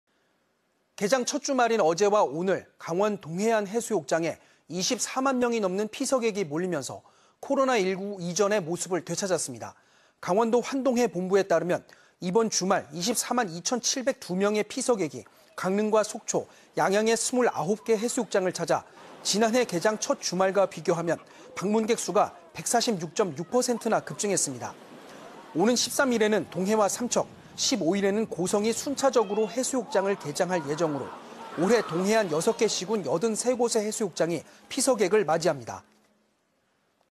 [단신] 강릉·속초·양양 해수욕장 개장 첫 주말 24만 명 몰려 220710
개장 첫 주말인 어제(9일)와 오늘
강원 동해안 해수욕장에
24만 명이 넘는 피서객이 몰리면서
코로나19 이전 모습을 되찾았습니다.

강원도 환동해본부에 따르면
이번 주말 24만 2천 702명의 피서객이
강릉과 속초, 양양의 29개 해수욕장을 찾아
지난해 개장 첫 주말과 비교하면
방문객 수가 146.6%나 급증했습니다.

오는 13일에는 동해와 삼척,
15일에는 고성이 순차적으로
해수욕장을 개장할 예정으로,
올해 동해안 6개 시군
83곳의 해수욕장이 피서객을 맞이합니다.